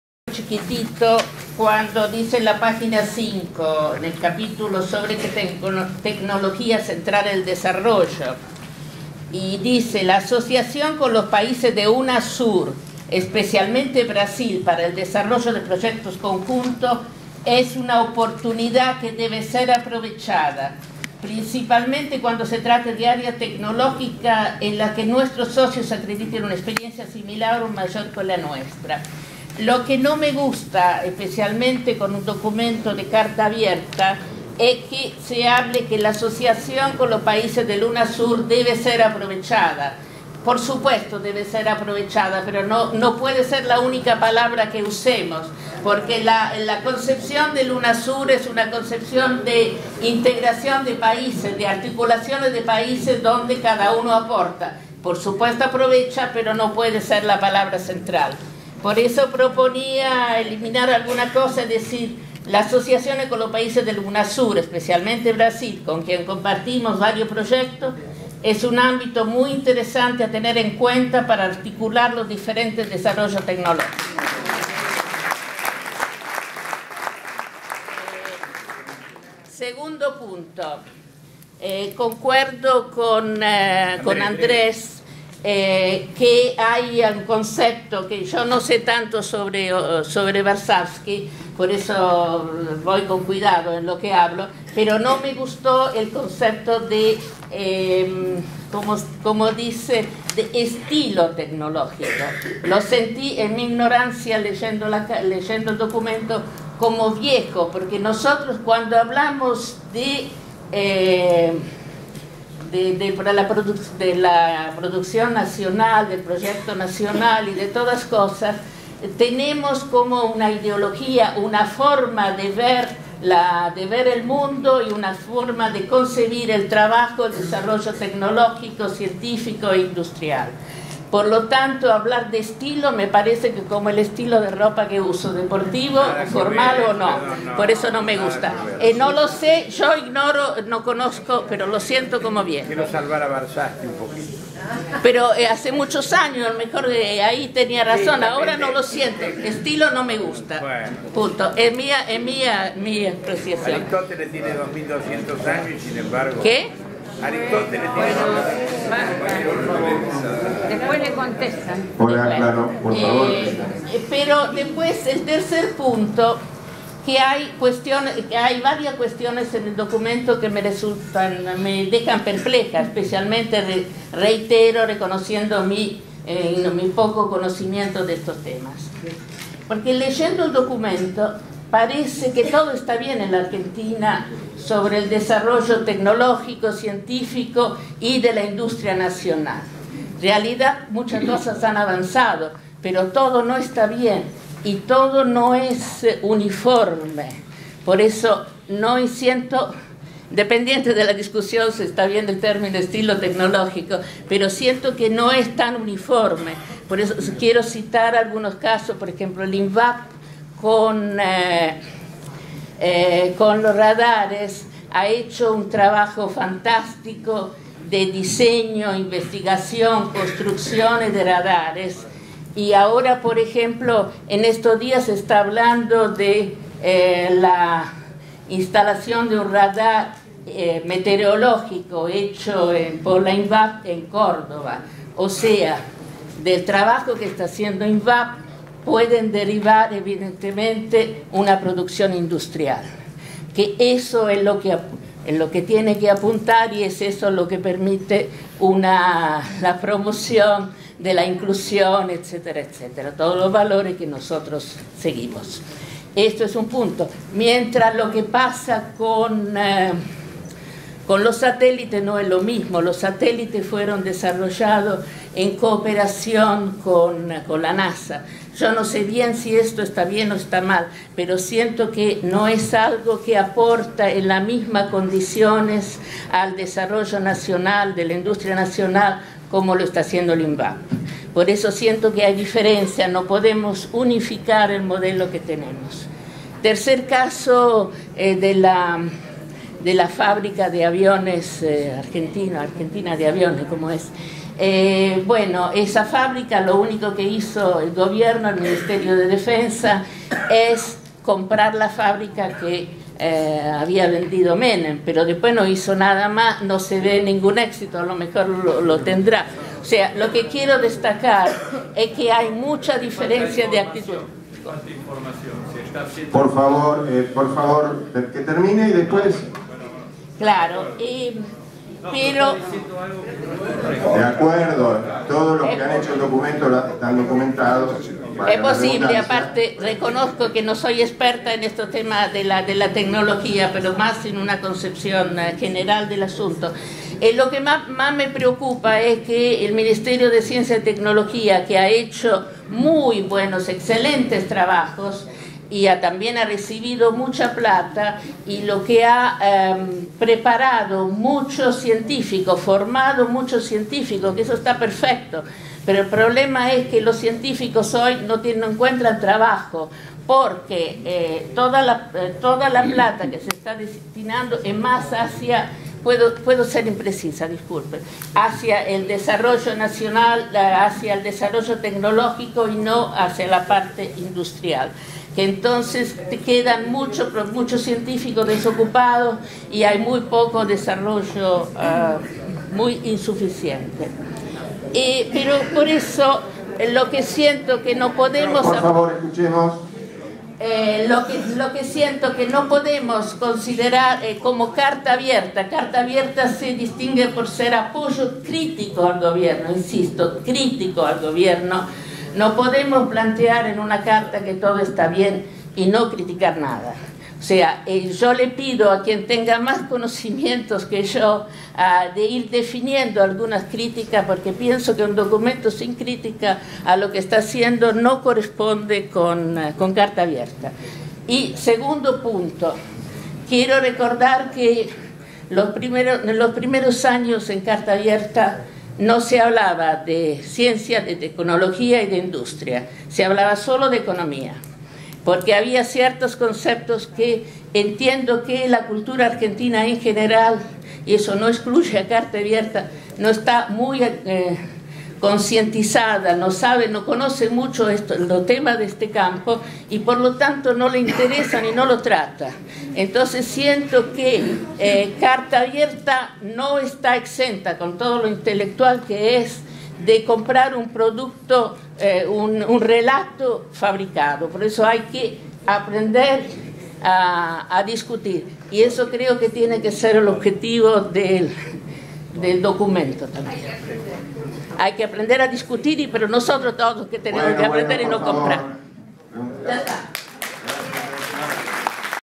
04CA Grazia Civinini / Tecnología
Discusión del documento "Proyecto Nacional de Desarrollo Inclusivo. Su Estilo Tecnológico", elaborado por la Comisión de Desarrollo Tecnológico de Carta Abierta. Asamblea del 11 de octubre de 2014. Biblioteca Nacional. Buenos Aires.